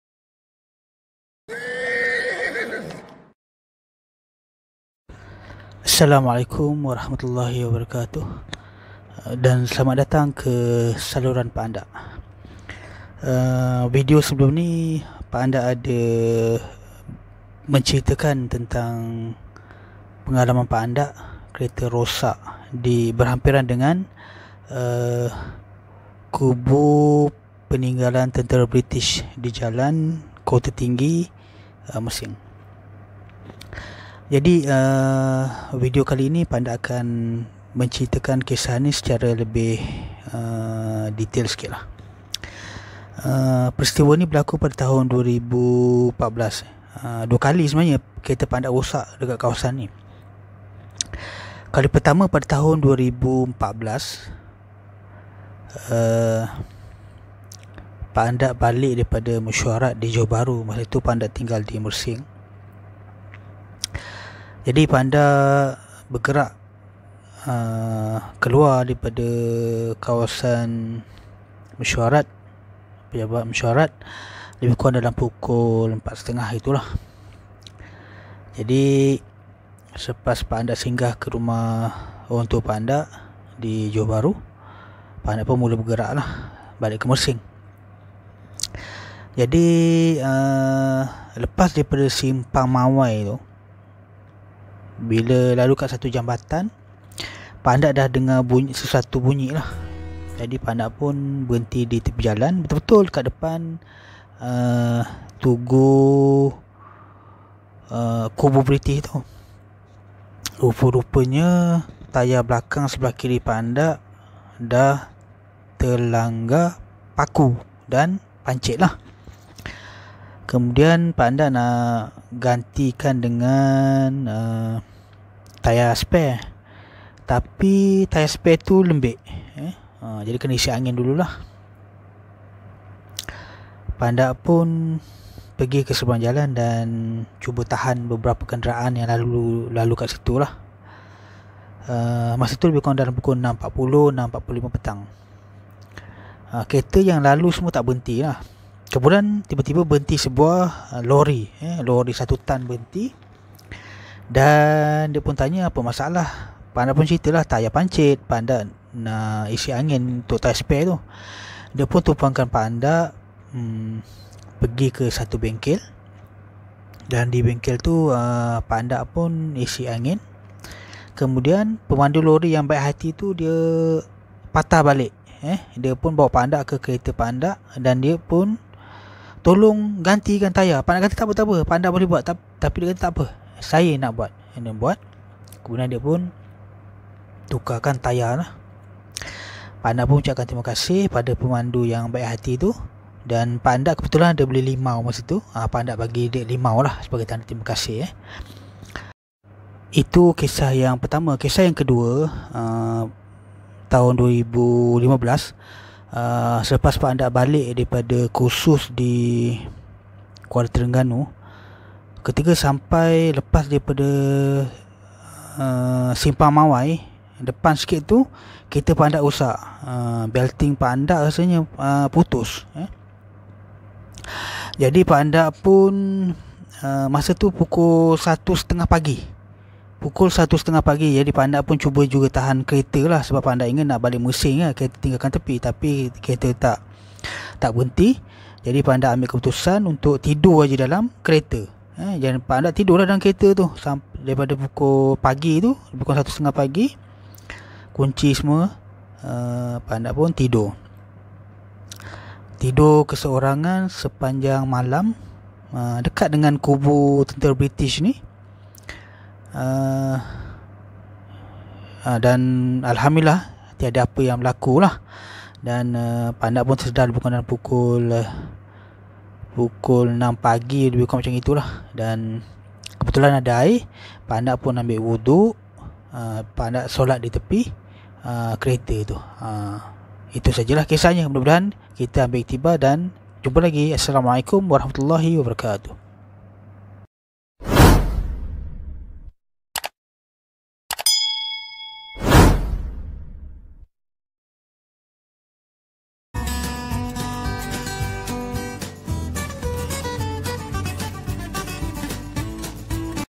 Pengalaman menyeramkan, kereta rosak pada jam 1.00 pagi dekat kubu British yang berhantu!!
Pengalaman yang tak boleh dilupakan.
Kereta rosak sebanyak dua kali berhampiran dengan kubu British yang dikatakan berhantu,
Kali kedua kereta rosak pada jam 1.00 pagi dan berseorangan dengan dikelilingi hutan sawit paling tak dapat dilupakan..